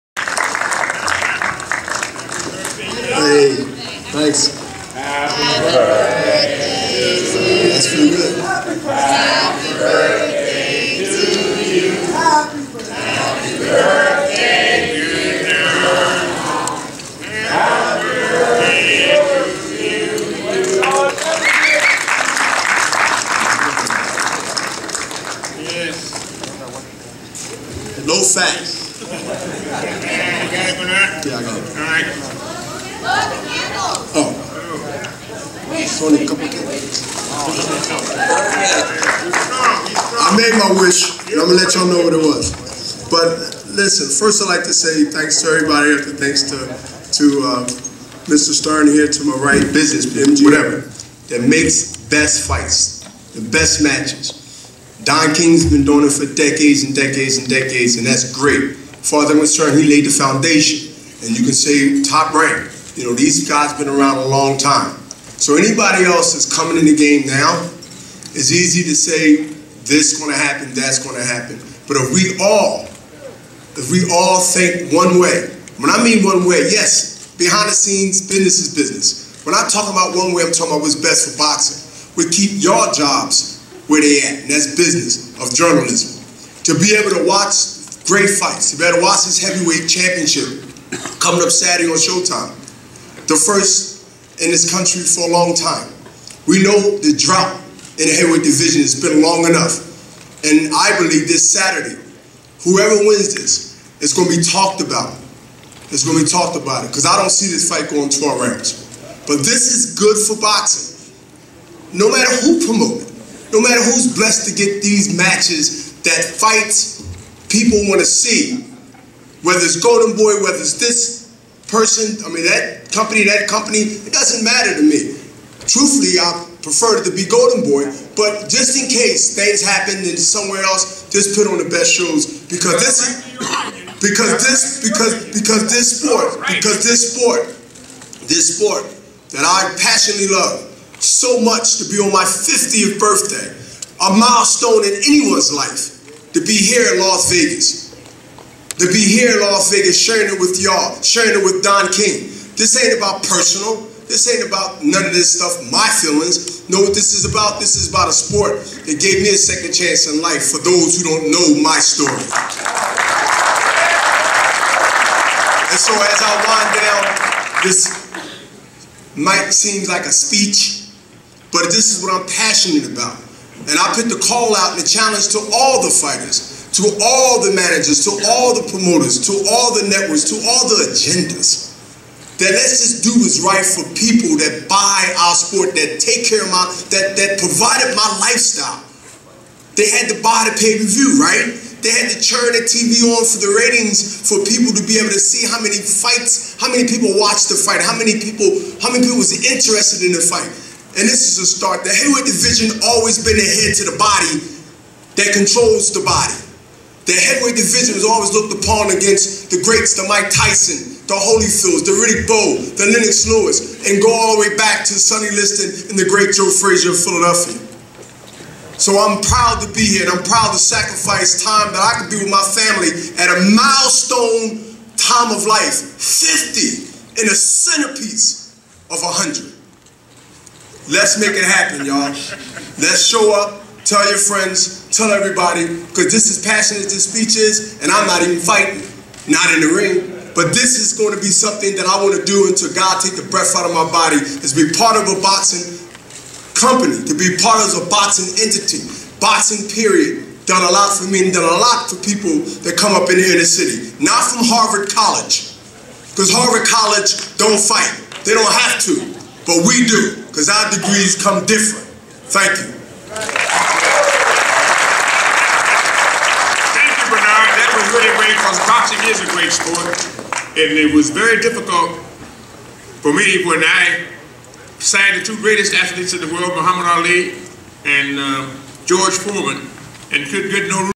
Bernard Hopkins celebrates 50, talks making great boxing matches again
Bernard Hopking talks turning 50 years old and how we can improve the sport, starting with making great matches.